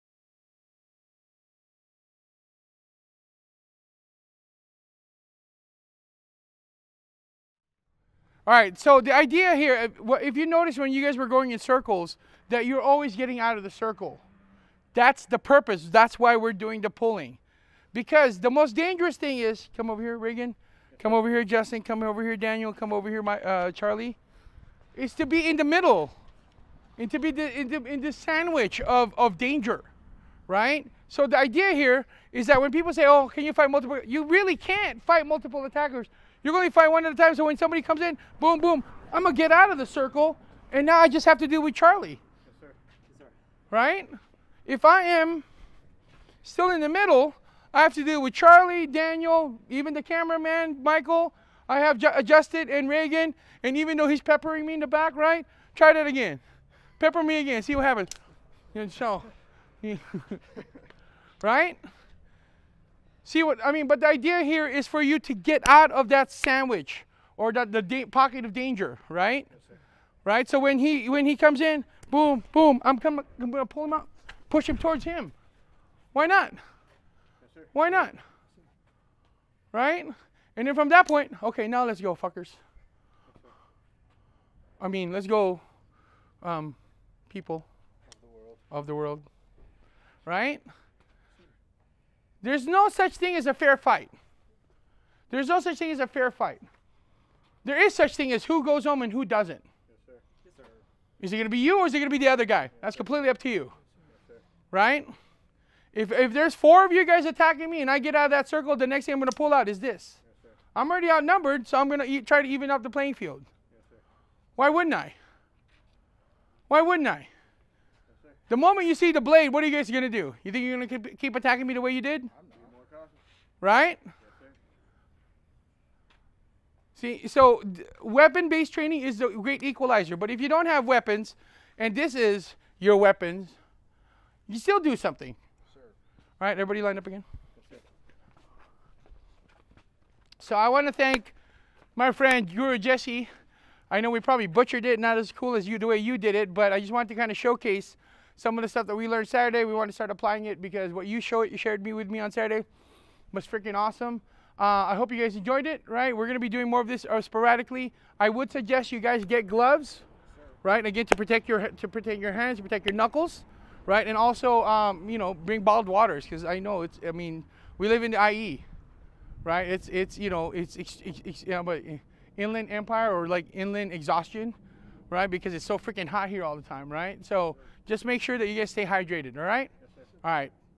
Putting all together
Now Guro Francis is demonstrating how he puts the entire series into application.
Thank you to Guro Jesse Santiago of @FMAPulse for showing us this Defensor Method Drill